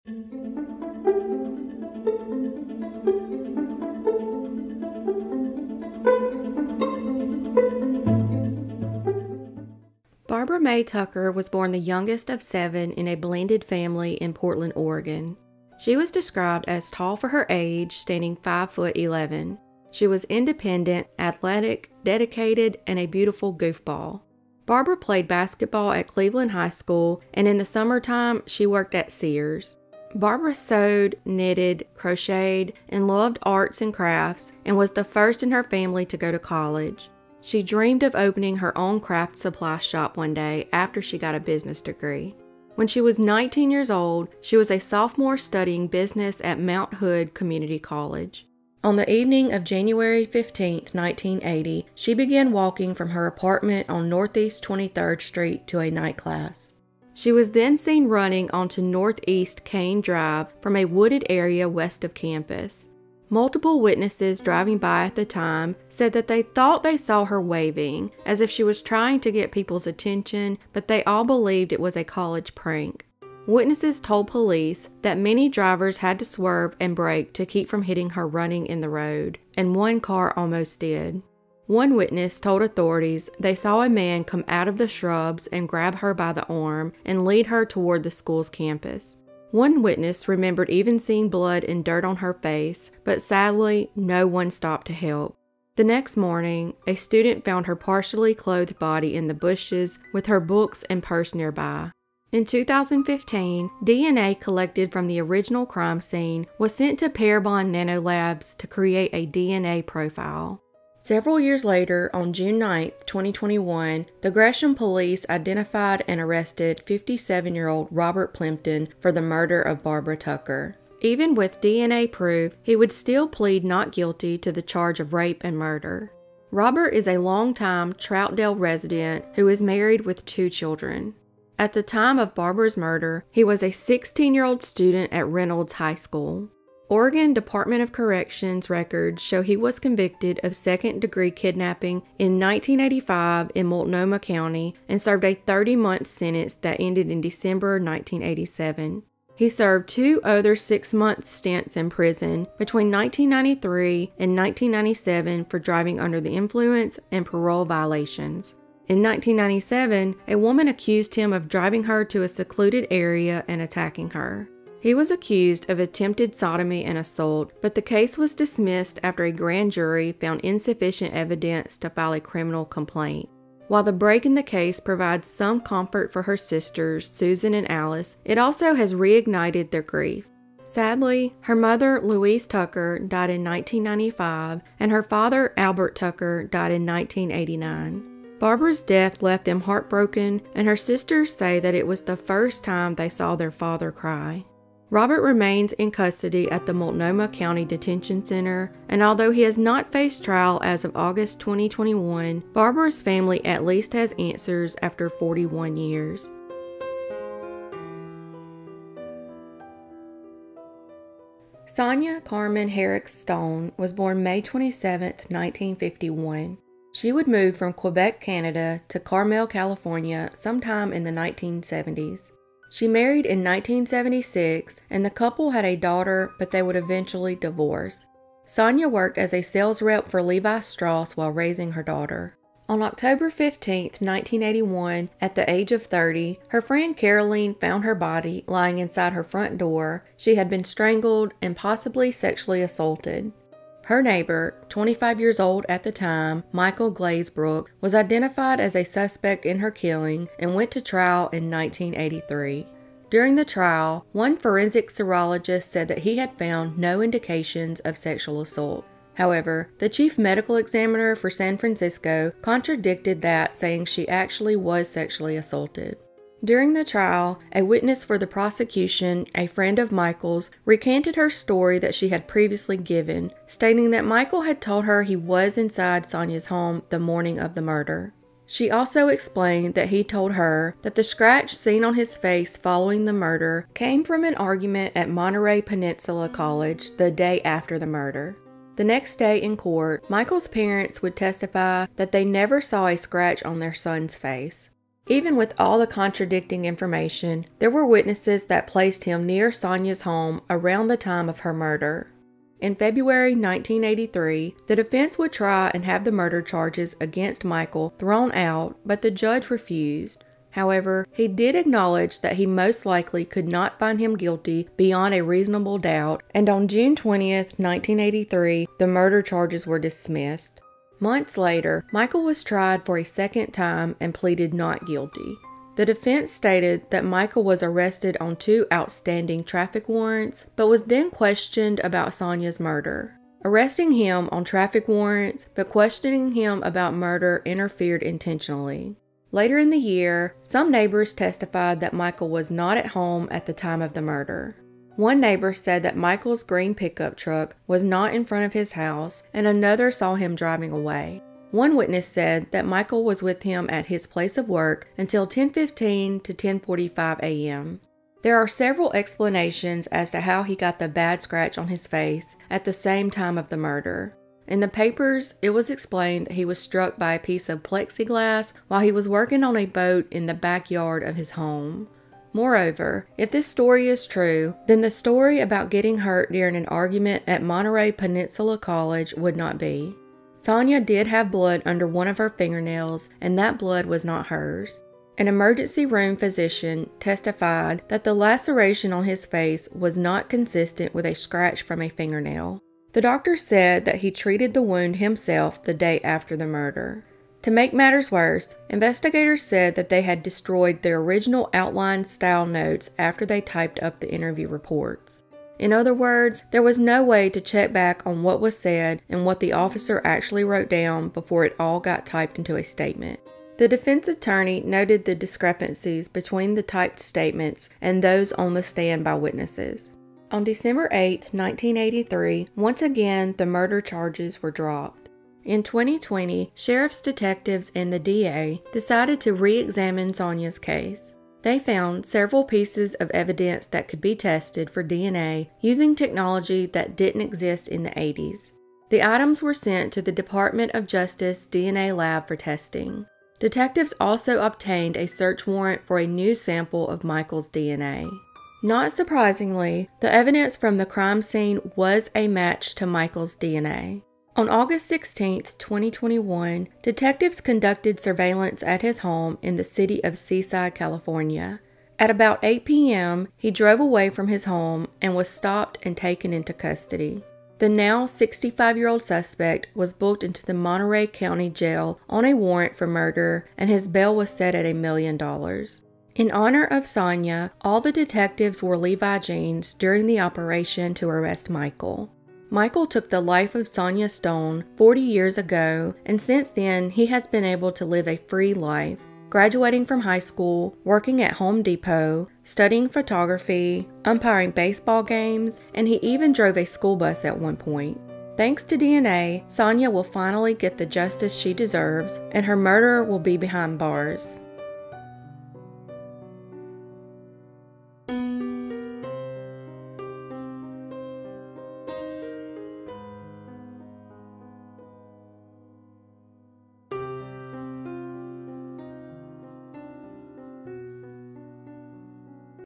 2 Cold Cases Solved After 40 Years
Barbara Mae Tucker (00:10) was murdered while motorist saw her in distress but thought it was a college prank and kept driving. The case was finally solved after 41 years. It took 40 years to convict Sonia Stone's (03:31) killer who was tried twice when the murder originally happened.